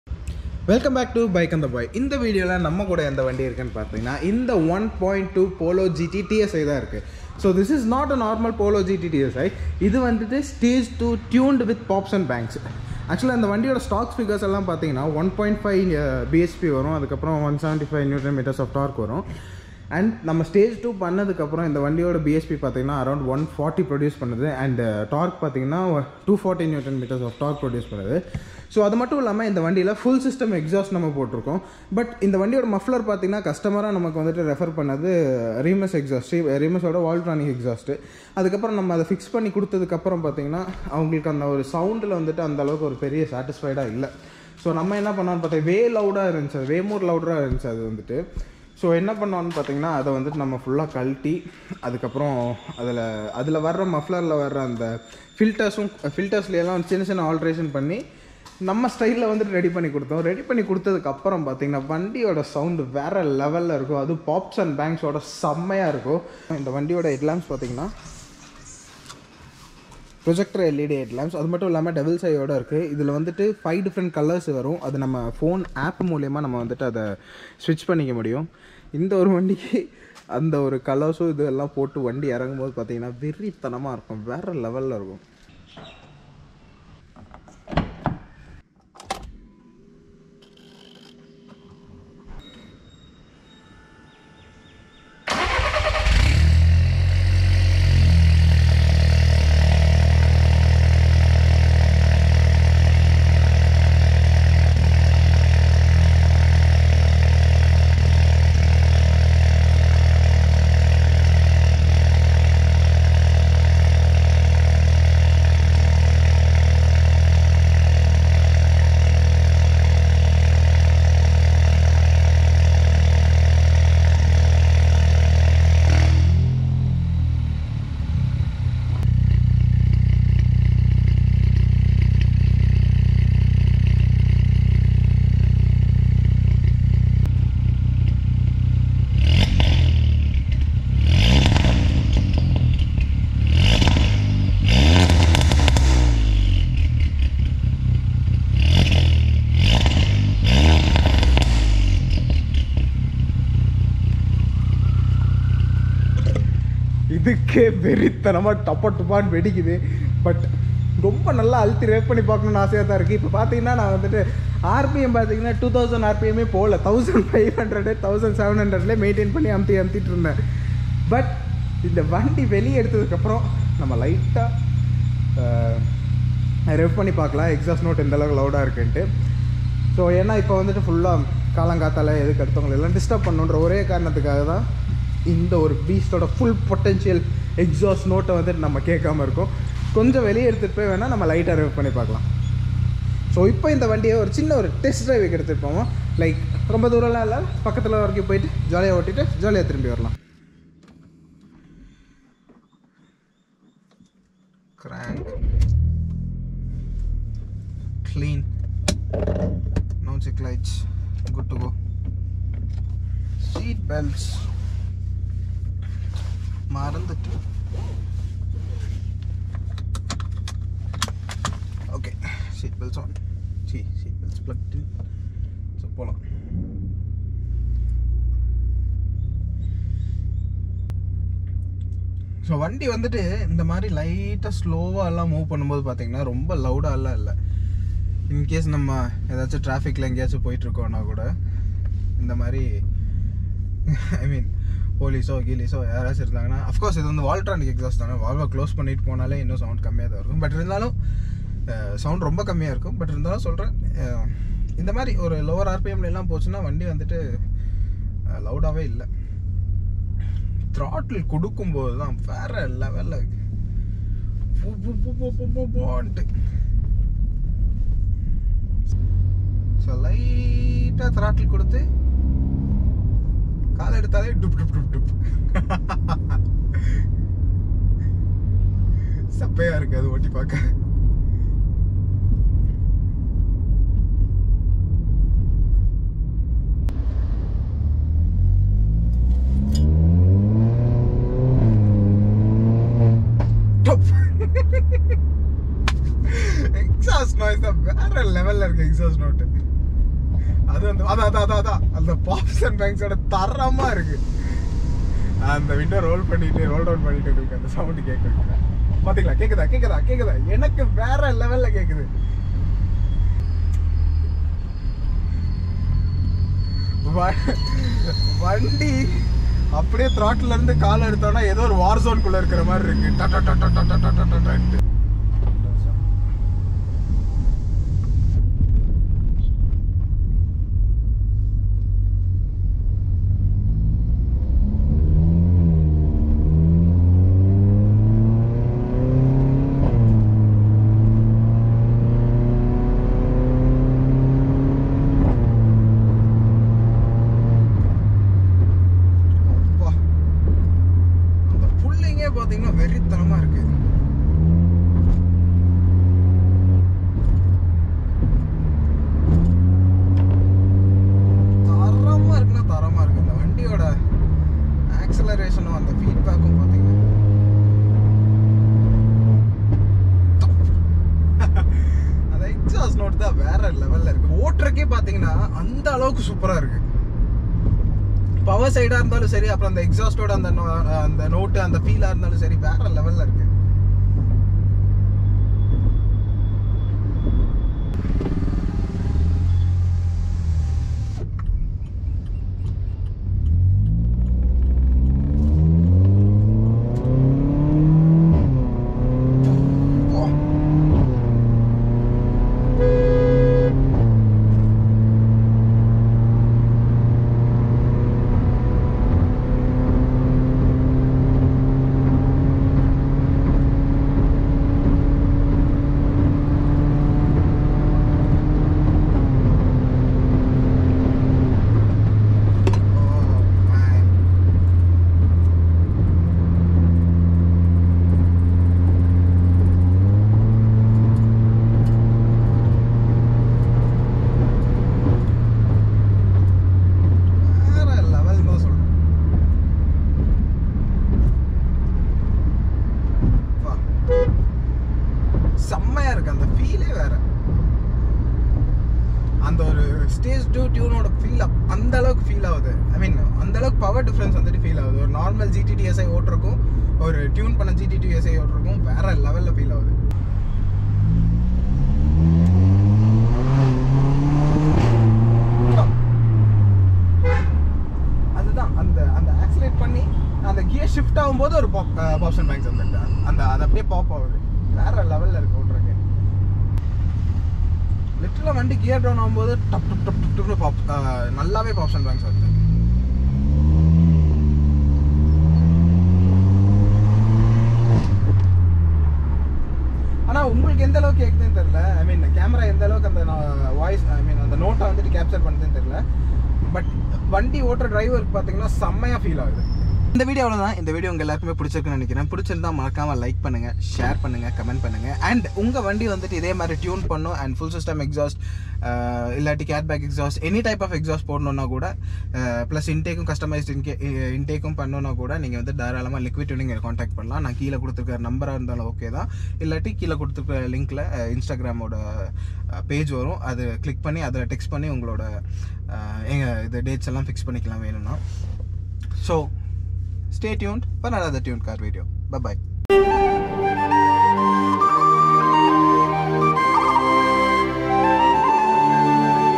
VW Polo GT TSI STAGE 2 # TUNED🔥 | stage2 | likwidtuned | BAB | Tamil
The Car shown in this video is a VW polo GT TSI, with a Stage 2 ECU tune with pops and bangs from Tuned To Race and a non resonated Exhaust system with a valvetronic Remus Muffler( which was reworked in Likwid Tuning)
STOCK 105BHP/175NM
TUNED 140BHP/240NM

To get your car tuned from Likwid Tuning

Thankyou